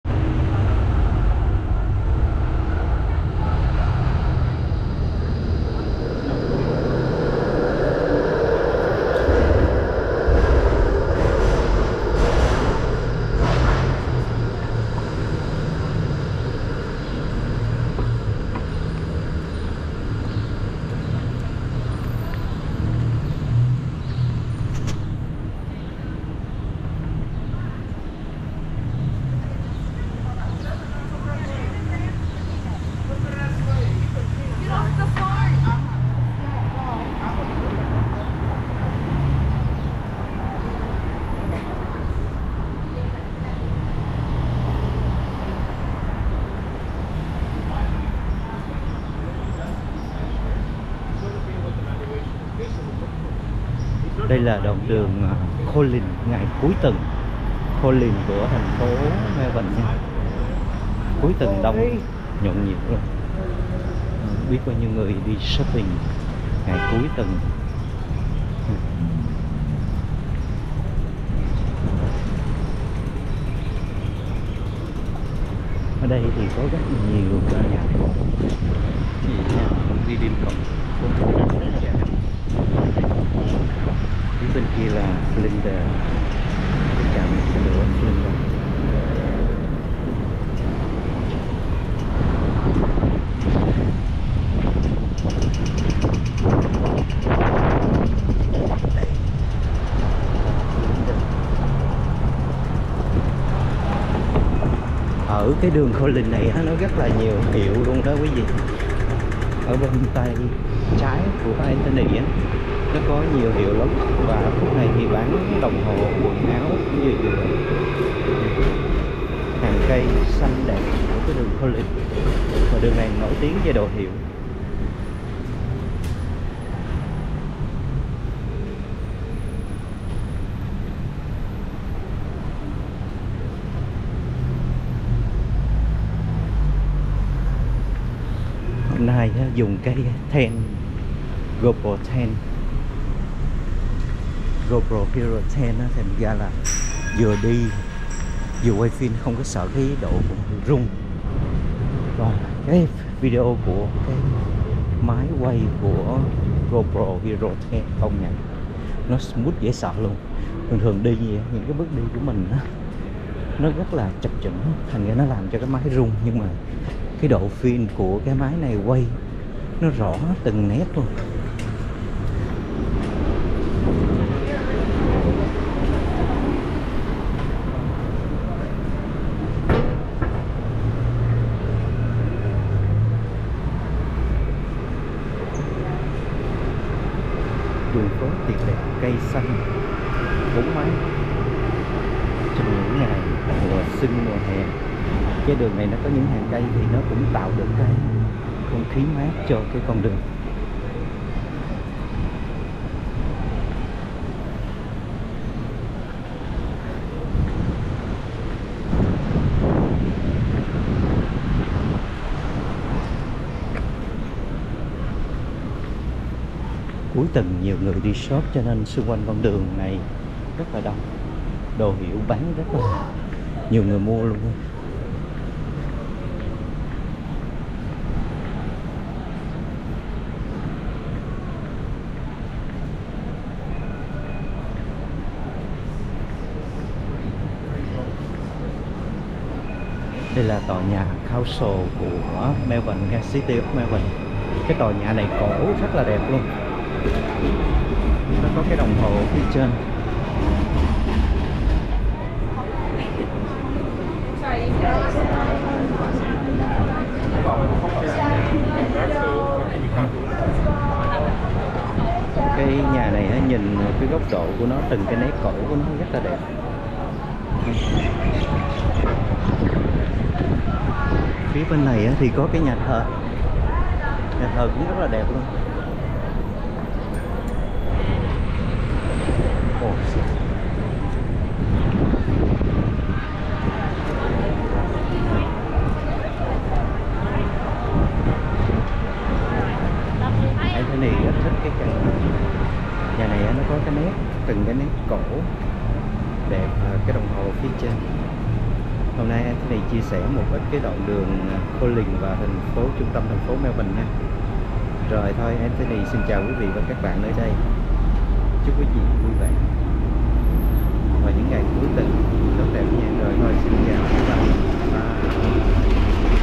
4K Đi bộ dọc theo hàng cây xanh đường Collins Mel Úc Châu – Weekend Melbourne Collins
4K Đi bộ dọc theo hàng cây xanh đường Collins Mel Úc Châu – Weekend Melbourne Collins